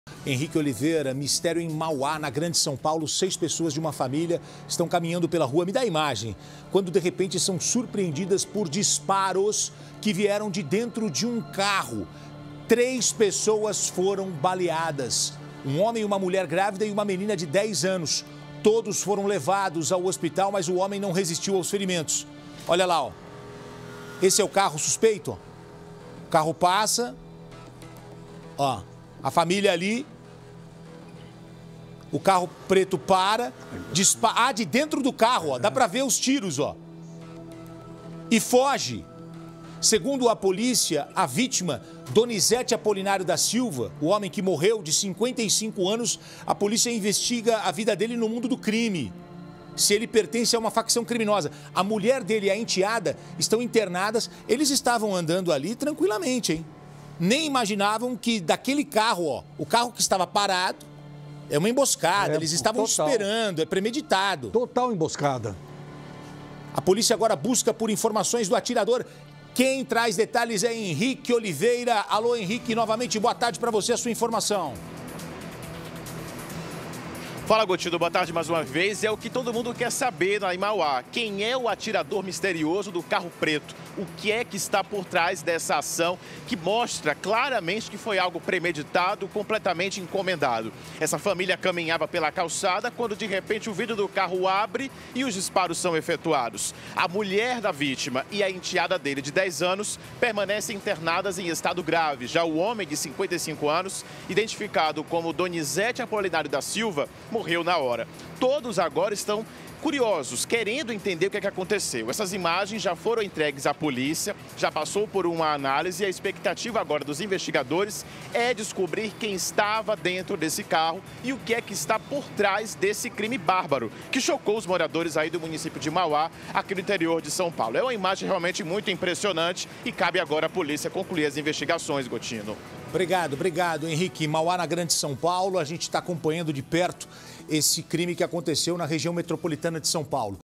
Família é atacada à tiros em Mauá
Seis pessoas de uma família em Mauá foram surpreendidas por desparos e três delas foram baleadas e uma morreu. A polícia investiga o envolvimento da vítima morta no mundo do crime e as outras estão no hospital, já a polícia investiga quem foi o assassino e quais suas motivações.